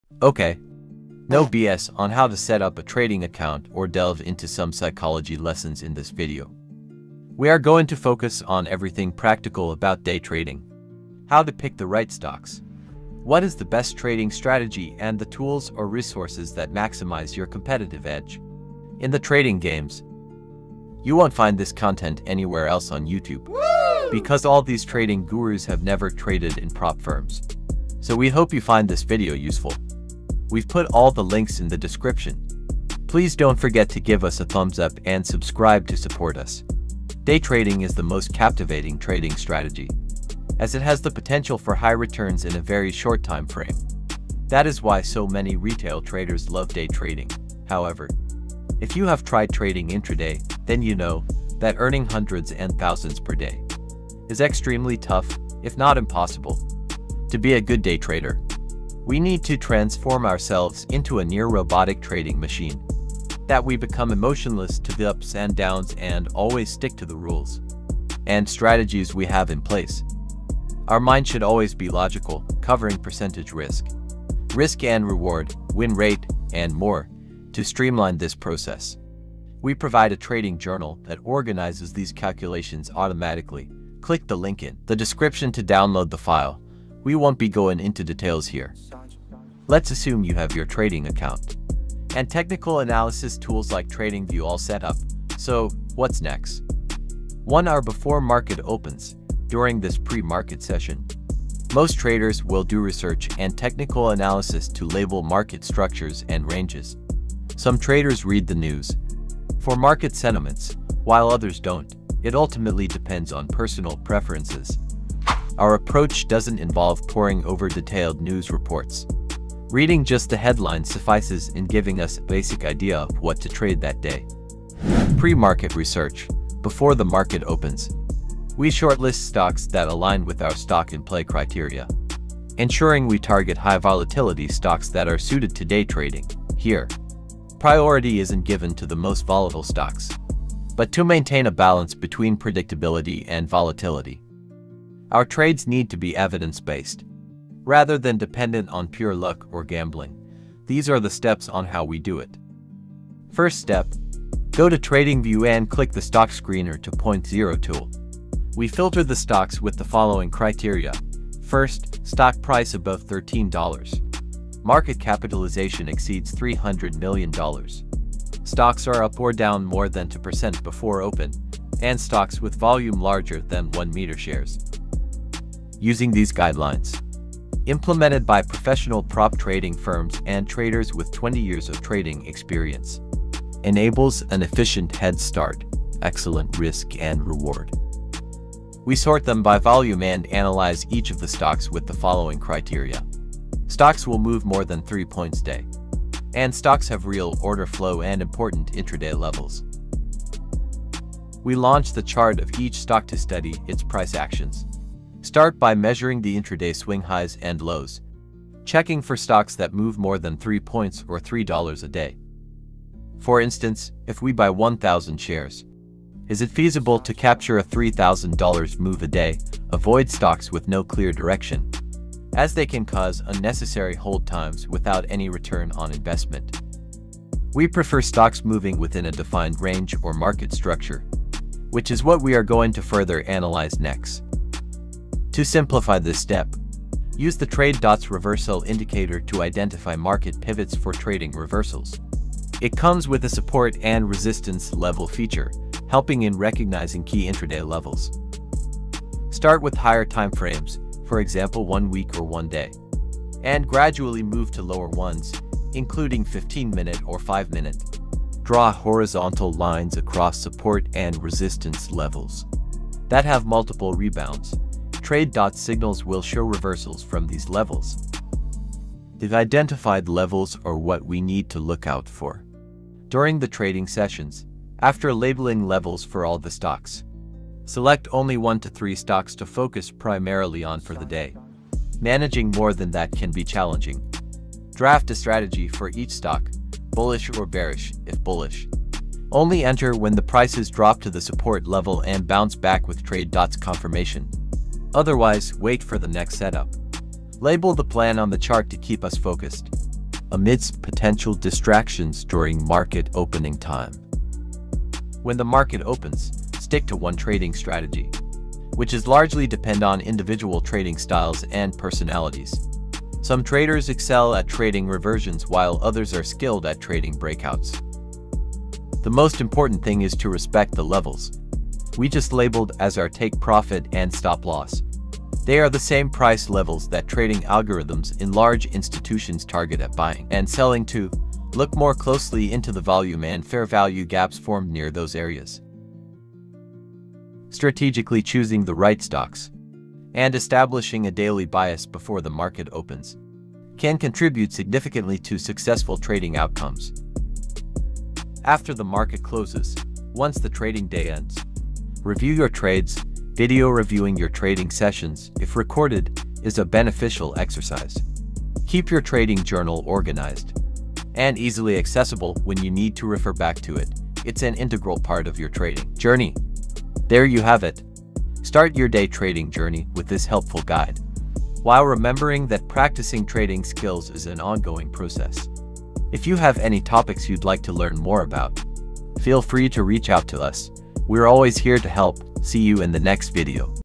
How to Start Day Trading 2024 (No BS, Strategies Only)
This video shows you a buy-the-dip trading strategy with only two indicators that help you identify the perfect entry on any high-growth stocks.

0:00 Introduction
0:37 About Day Trading
1:37 1 Hour Before Market Opens
2:33 Stock in Play Filtering Strategy
3:47 Easy Technical Analysis Strategy
5:05 Entry Strategy When Market Opens
5:48 Take Trading Journal After Market Closes
6:06 Outro